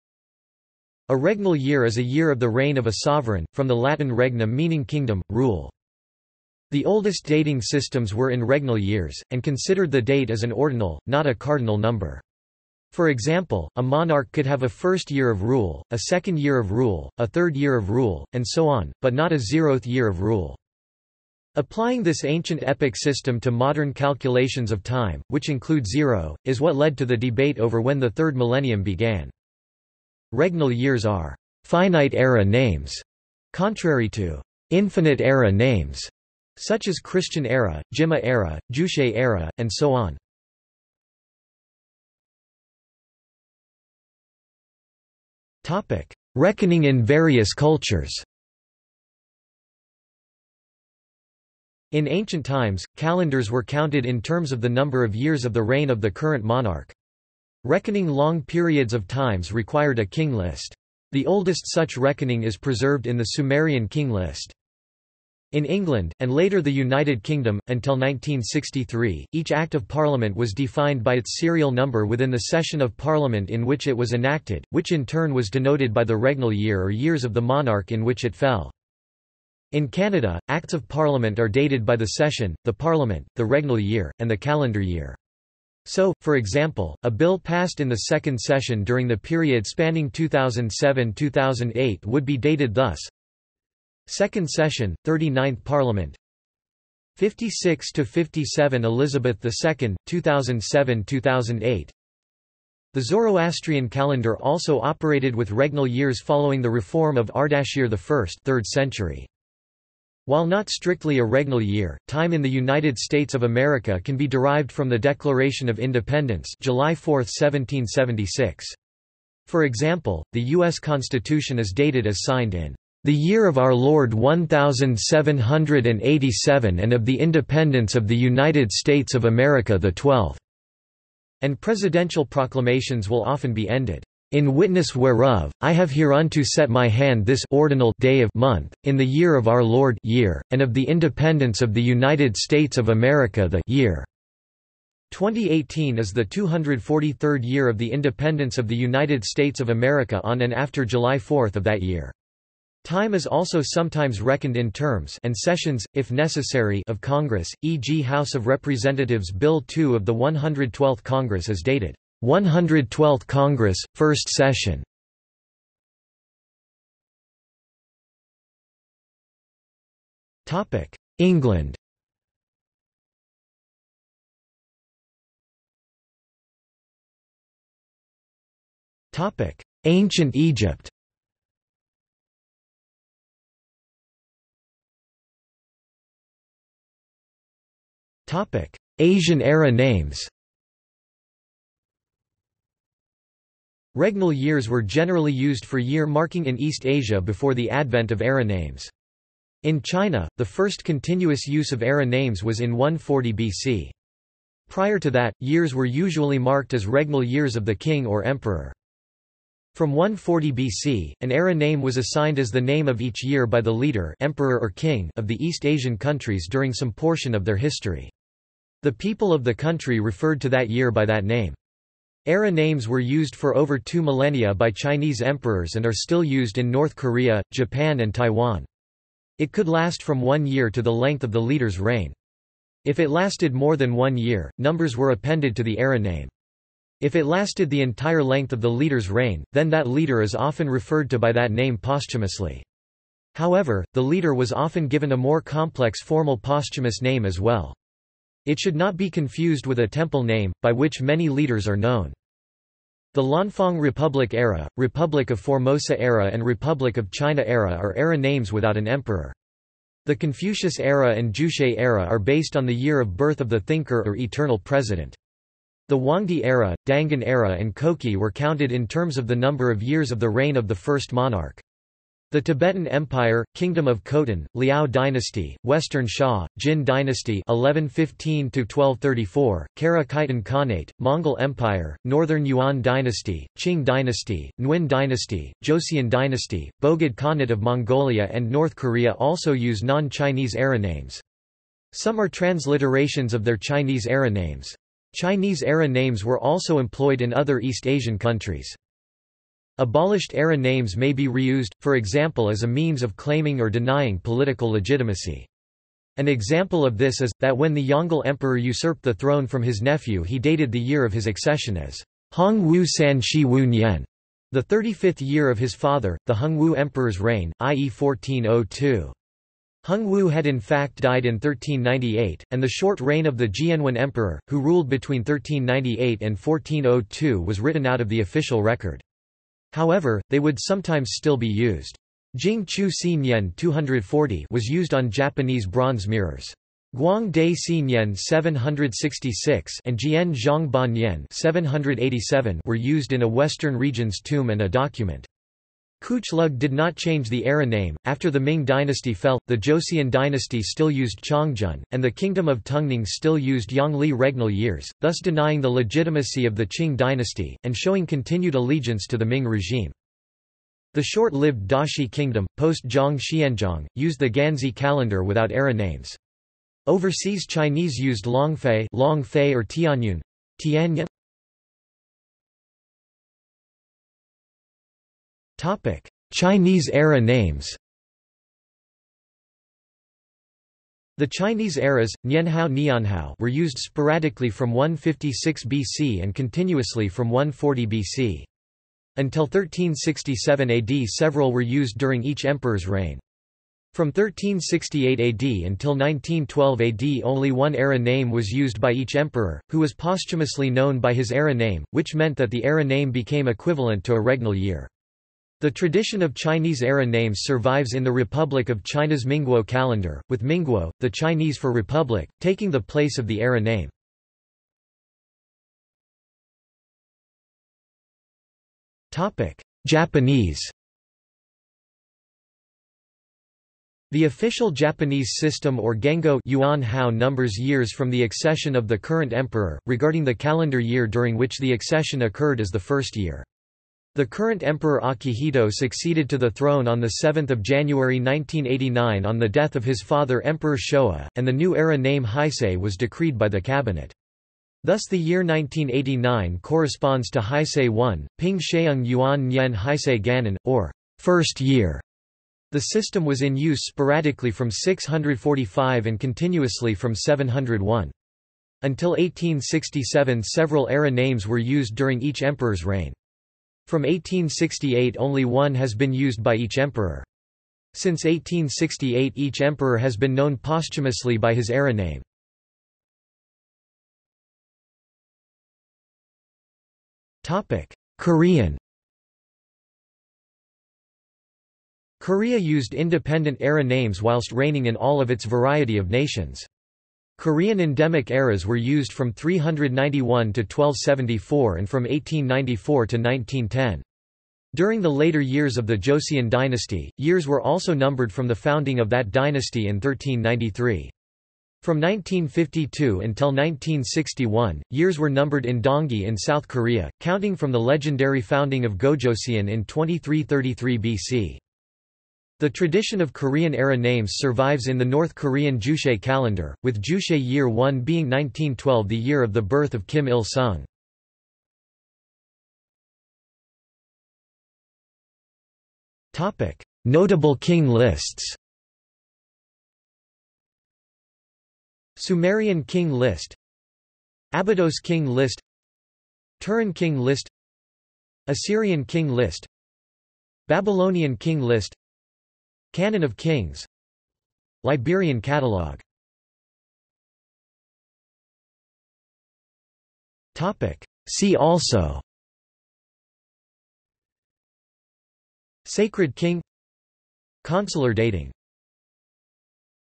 Regnal year | Wikipedia audio article
This is an audio version of the Wikipedia Article:
Regnal year

00:00:42 1 Reckoning in various cultures
00:02:46 2 England
00:02:55 3 Ancient Egypt
00:03:04 4 Asian era names
00:06:12 4.1 Chinese era names
00:06:55 4.2 Japanese
00:07:54 4.3 Korean
00:08:42 5 Notable king lists
00:09:06 6 See also

- Socrates

SUMMARY
A regnal year is a year of the reign of a sovereign, from the Latin regnum meaning kingdom, rule.
The oldest dating systems were in regnal years, and considered the date as an ordinal, not a cardinal number. For example, a monarch could have a first year of rule, a second year of rule, a third year of rule, and so on, but not a zeroth year of rule.
Applying this ancient epoch system to modern calculations of time, which include zero, is what led to the debate over when the third millennium began.
Regnal years are "finite era names", contrary to "infinite era names" such as Christian era, Jimmu era, Juche era, and so on.